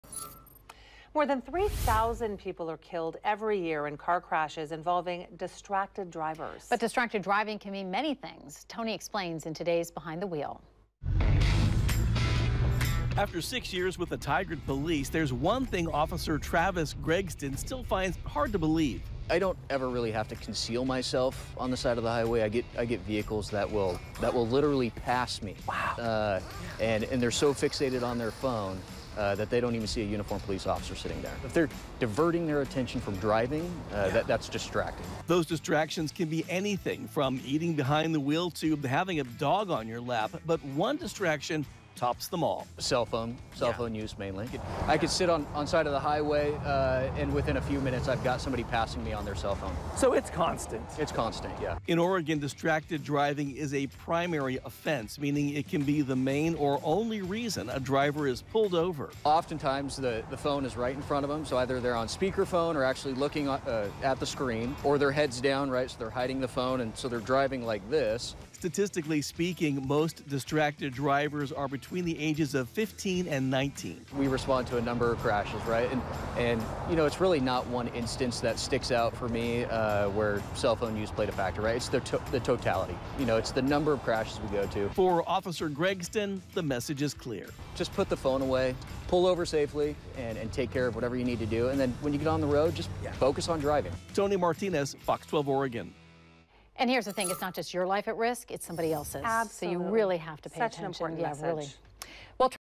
Behind the Wheel: Distracted driving
More than 3,000 people are killed each year in crashes involving distracted drivers.